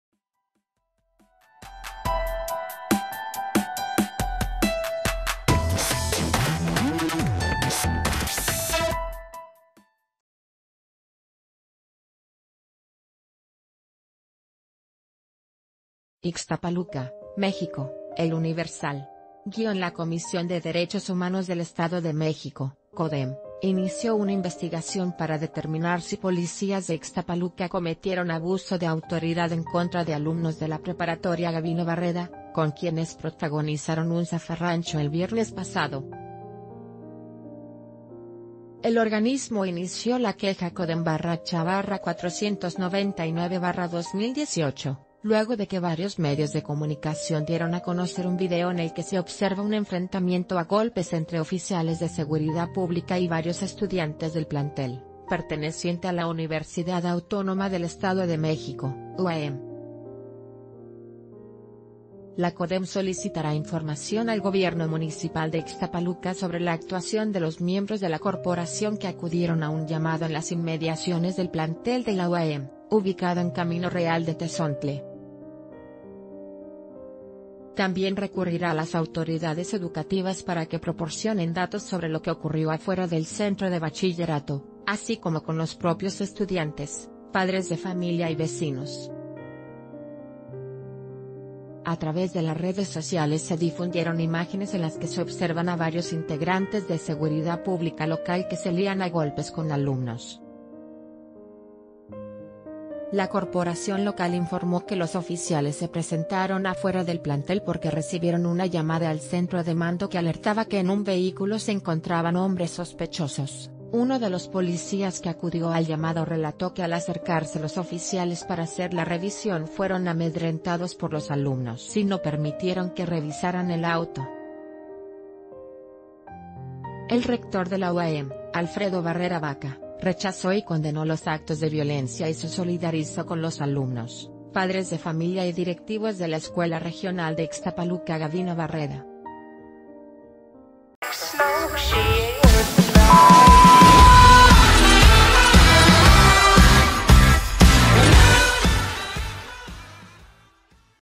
Indagan presunto abuso por parte de policías de Ixtapaluca - El Diario de Yucatán
IXTAPALUCA, México (El Universal).- La Comisión de Derechos Humanos del Estado de México (Codhem) inició una investigación para determinar si policías de Ixtapaluca cometieron abuso de autoridad en contra de alumnos de la preparatoria Gabino Barreda, con quienes protagonizaron un zafarrancho el vier...